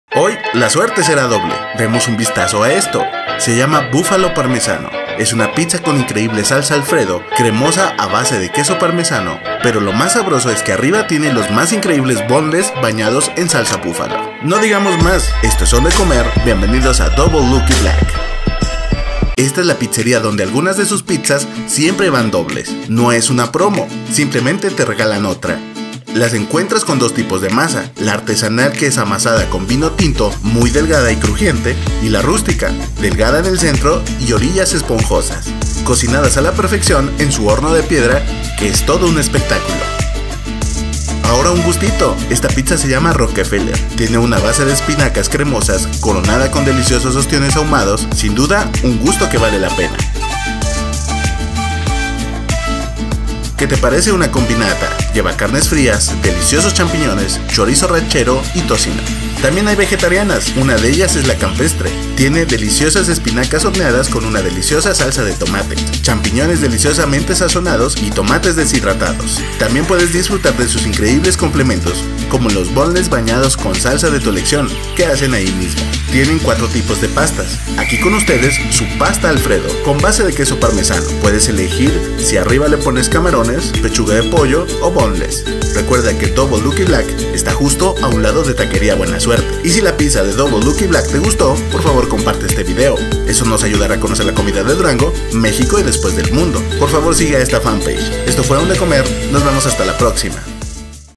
Double Lucky, Durango, Dgo.
"Double Lucky" es un lugar de pizzas doblemente exquisitas, por que algunas de sus pizzas siempre van dobles, recuerda no es una promo simplemente te regalan una. No se lo pierdan Tragonautas amantes de la pizza.

Están ubicados en Blvd. Domingo Arrieta #308 Col. El Refugio a un costado de Tacos Buena Suerte